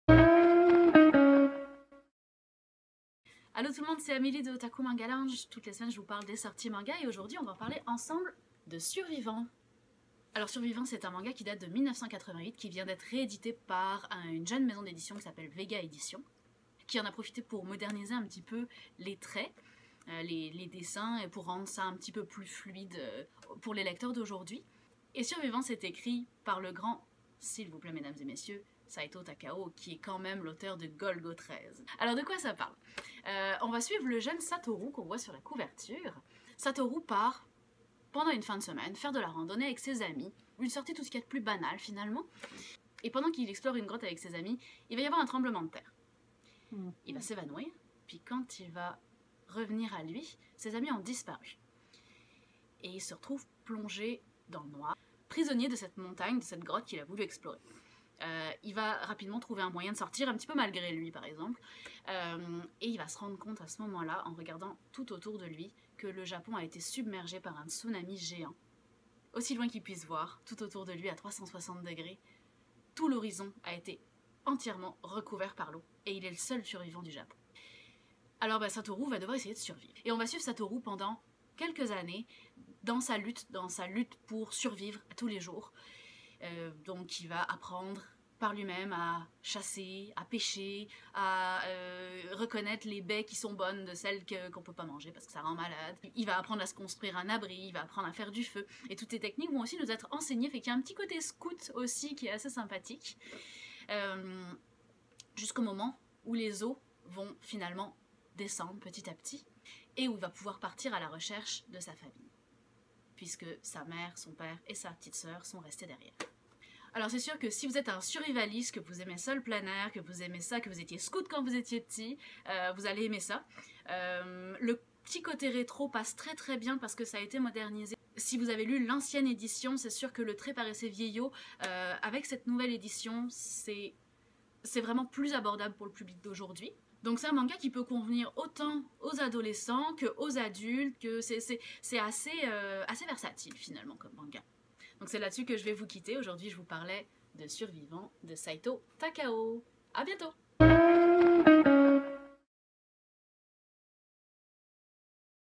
La mangathèque O-Taku: Survivant, de Saito Takao - seinen thriller #2
Amé de O-Taku Manga Lounge vous parle du manga Survivant, par Saito Takao, auteur de Golgo 13, sorti cette semaine au Québec